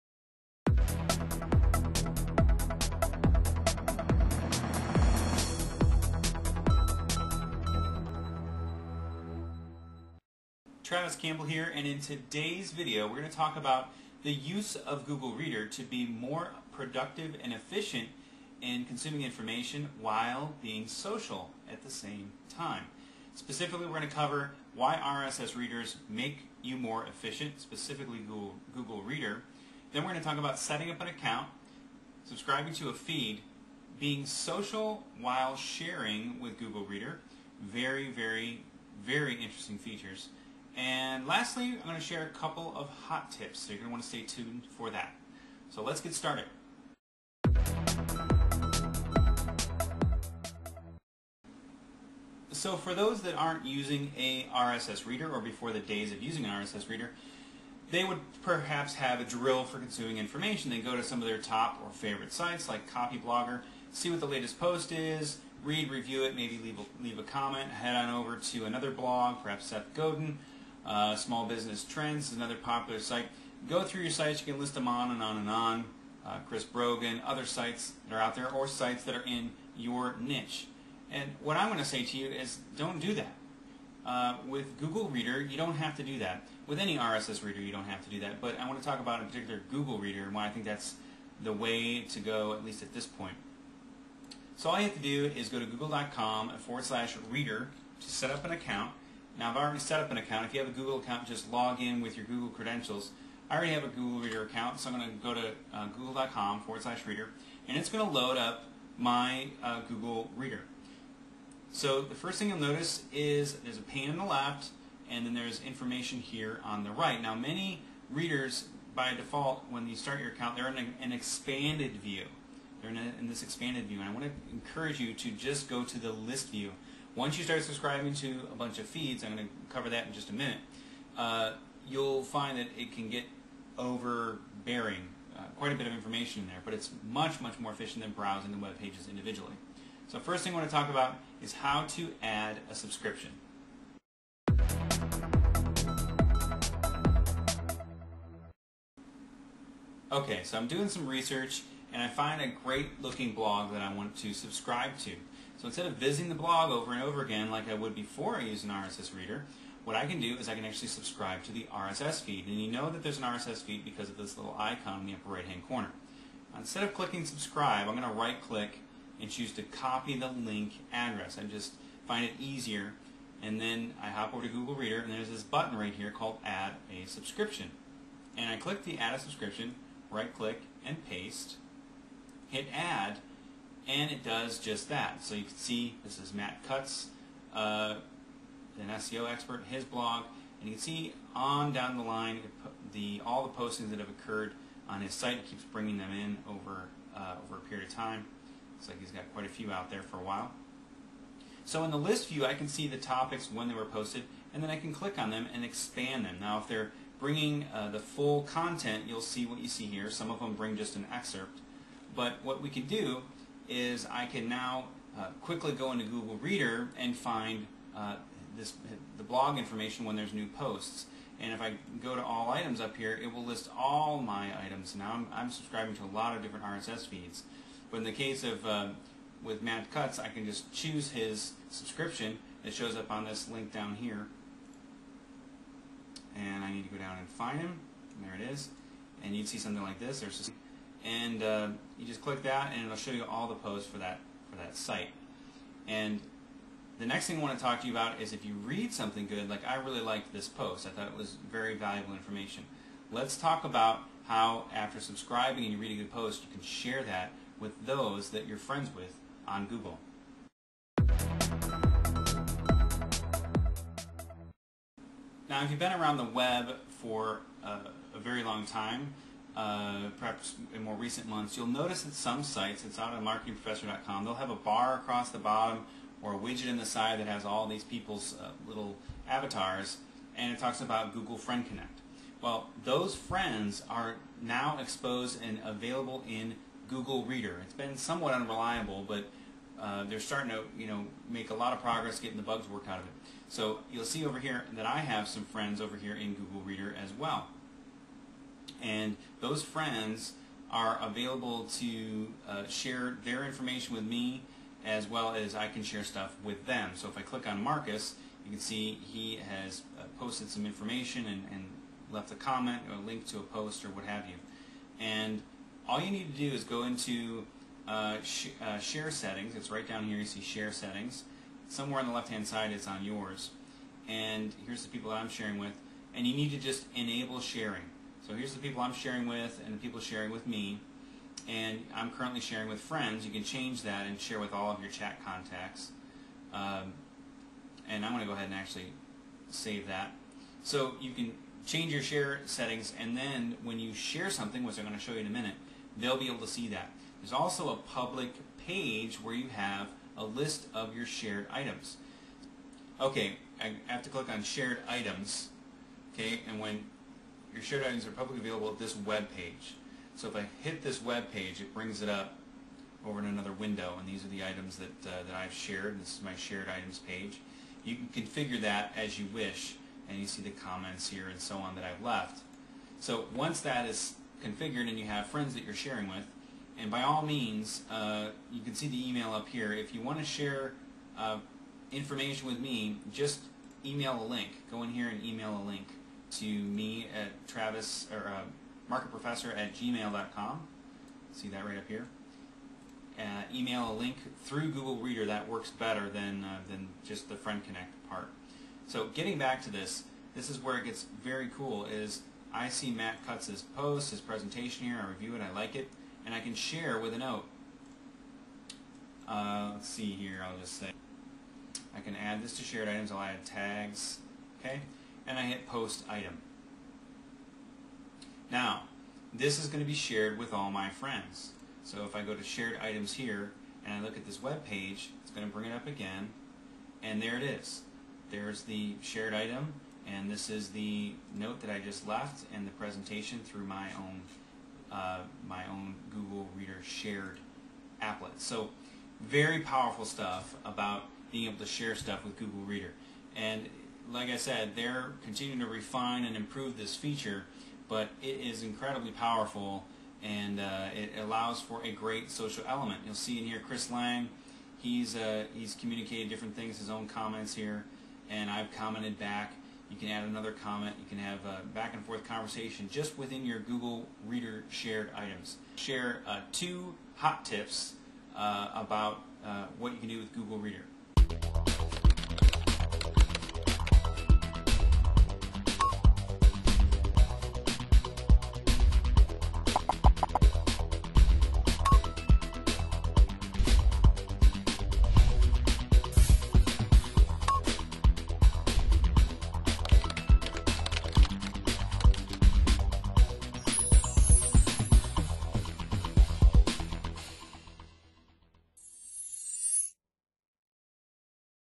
Google Reader Social Networking-Part 1
Being social and sharing with Google Reader.
1.) Why Use an RSS feed?
2.) Setting up your Google Reader account.
4.) Being social while sharing with Google Reader.
   -How to share with someone
   -Your Shared Items web page
   -Share items with me :-)
In part 2, I'll be sharing
5.) Two Hot Tips...(you don't want to miss this).

If you liked this video, and maybe learned a thing or two, please:
1.) Rate
2.) Favorite
4.) Comment.

Produced for viewers like you on 6/12/2009